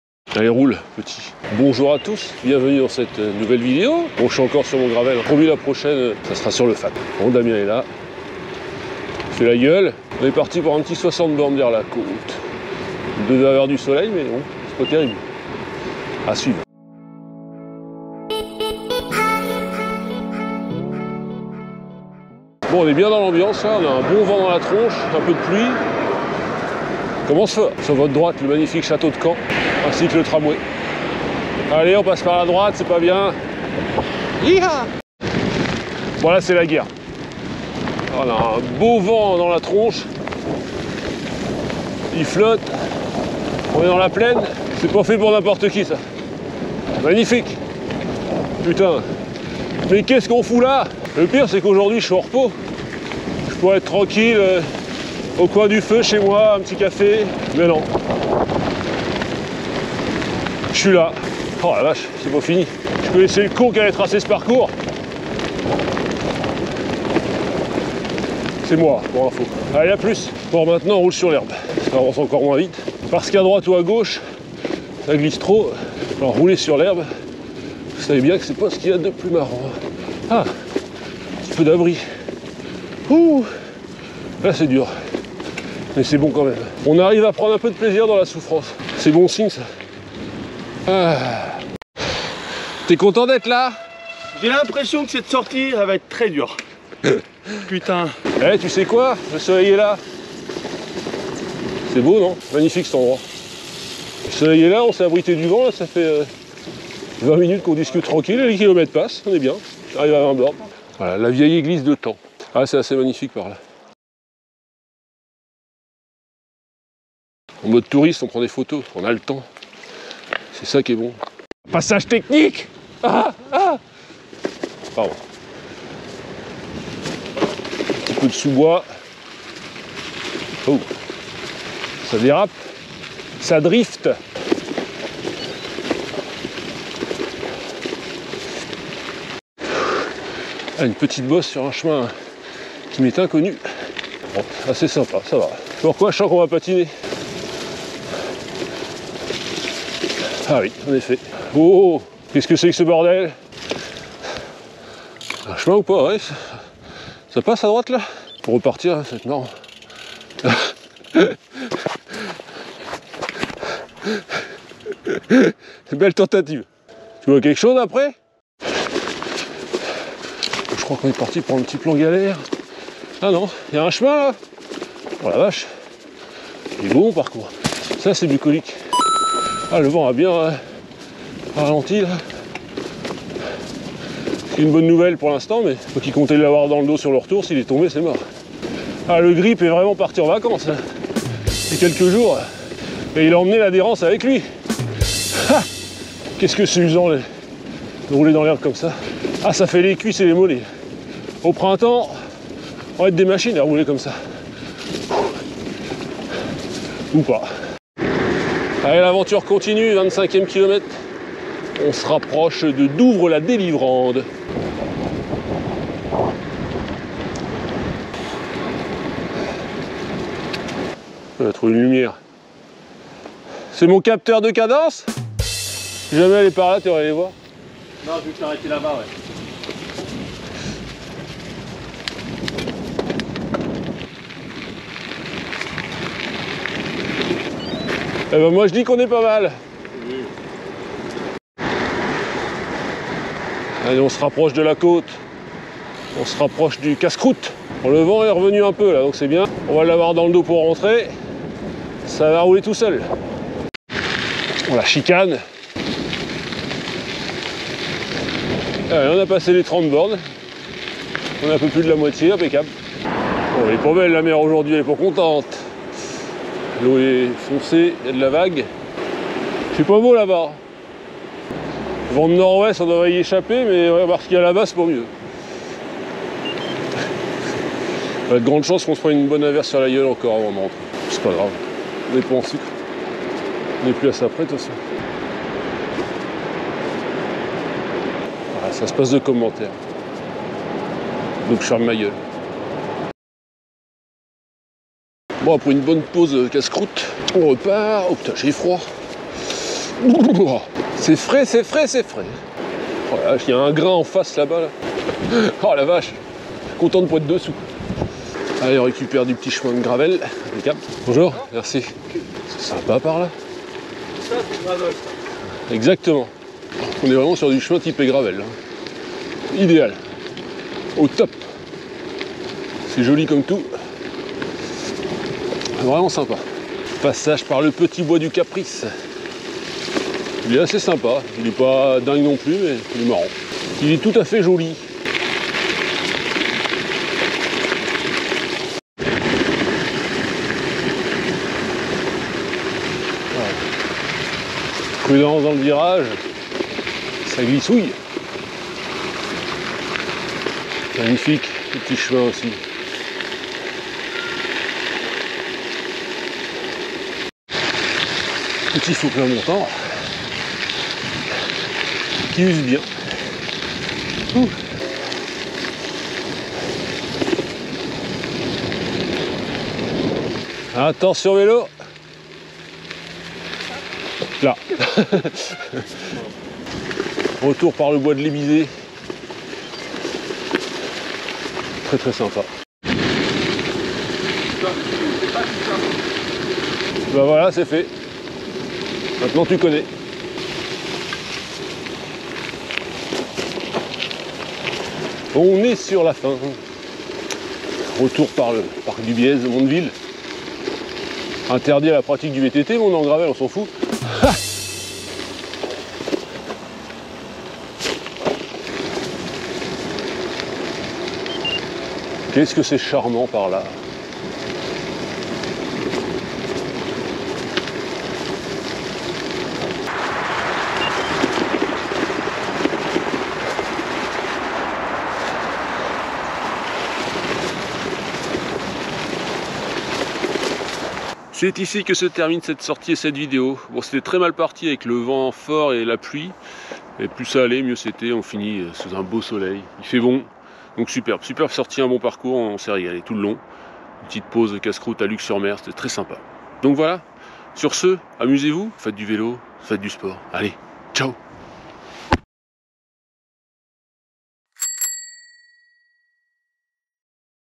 Le pire, puis le meilleur ! Belle sortie gravel
Encore une sacrée balade avec Damien, ça part très mal niveau condition climatique et parcours, mais la seconde partie est à l'opposé !! Du bon gravel !